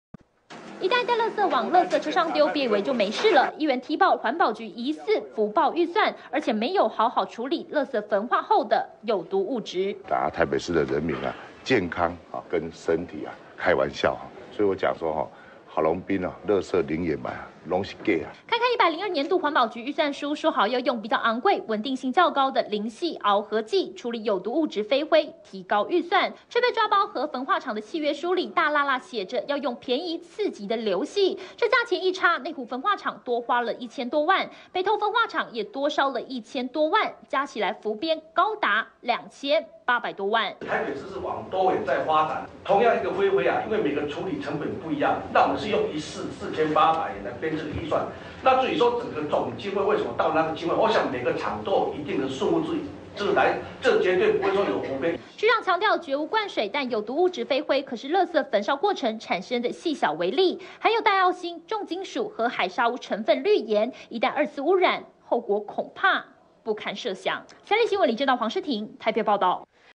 北市府圖利？處理「灰飛」浮編2800萬(三立新聞)
郝龍斌垃圾零掩埋，攏係假？！民進黨台北市議員踢爆，環保局疑似浮報預算，沒有好好處理垃圾焚化後有毒物質！攤開環保局102年預算書，說好要用比較昂貴穩定性較高的「磷性」螯合劑處理有毒物質「灰飛」，其含戴奧辛、重金屬、和海砂屋氯鹽，卻被抓包和合約書裡寫的便宜次級的「硫性」螯合劑，內湖焚化廠整整價差1111萬、北投焚化廠價差1784萬，兩者加起來灌水高達2800萬！